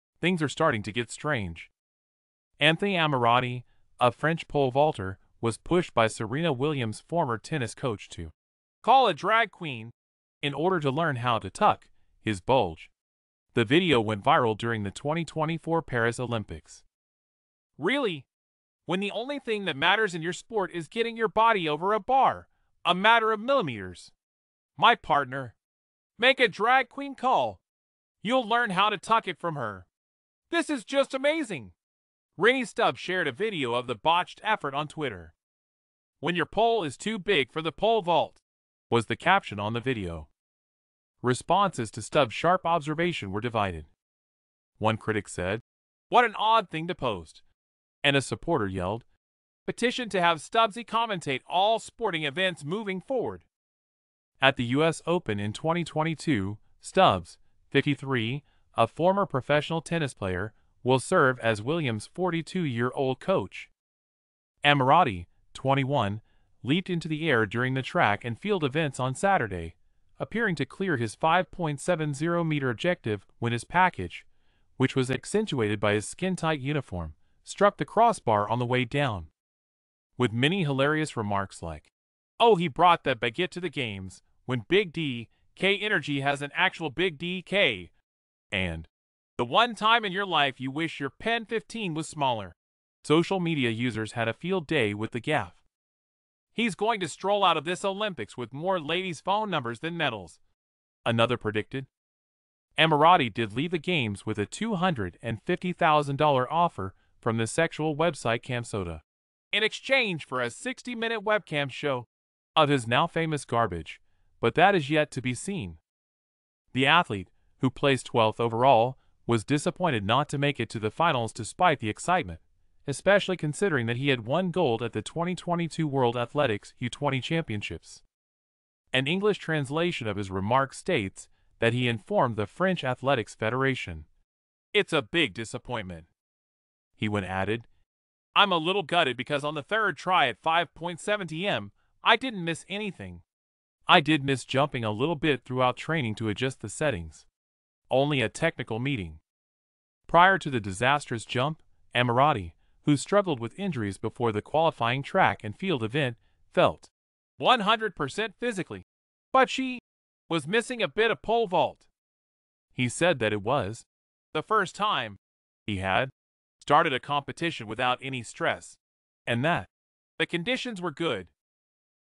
French pole vaulter Anthony Ammirati is advised to "call a drag queen" to learn how to "tuck" his...
Things are starting to get strange.
Anthony Ammirati, a French pole vaulter, was pushed by Serena Williams' former tennis coach to “call a drag queen” in order to learn how to “tuck” his bulge. The video went viral during the 2024 Paris Olympics.